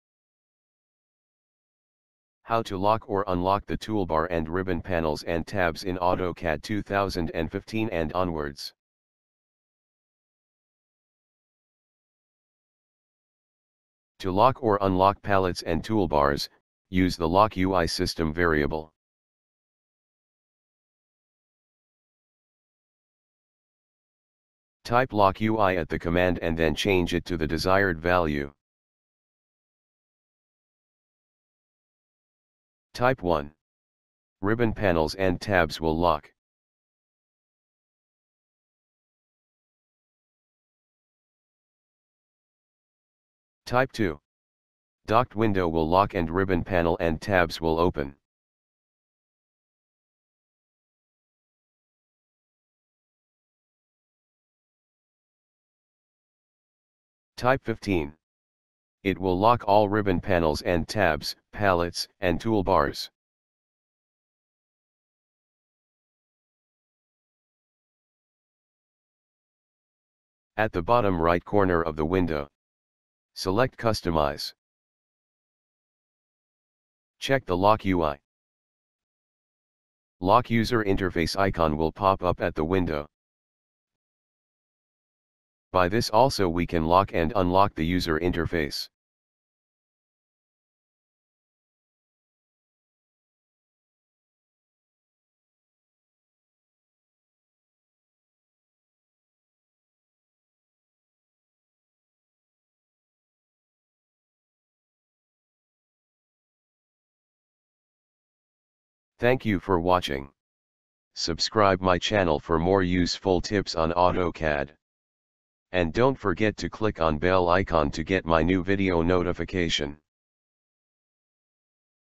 how to lock and unlock user interface and ribbon panels, tabs.
Hai Friends.
Welcome To UniquE CAD DesigN.

Thanks & Regards.
Harish M
UniquE CAD DesigN.

How to lock or unlock the user interface in AutoCAD

In AutoCAD 2014 and prior versions, you can lock or unlock the palettes and toolbars using the lock icon on the AutoCAD taskbar. This feature is not found in releases in 2015 and onward.

This is as designed. The Lock User Interface (LOCKUI) icon has been removed starting with AutoCAD 2015.

To lock or unlock palettes and toolbars, use the LOCKUI system variable.  Type LOCKUI at the command prompt and then change it to the desired value:

0 = Toolbars and windows not locked
1 = Docked toolbars locked
2 = Docked or anchored windows locked
4 = Floating toolbars locked
8 = Floating windows locked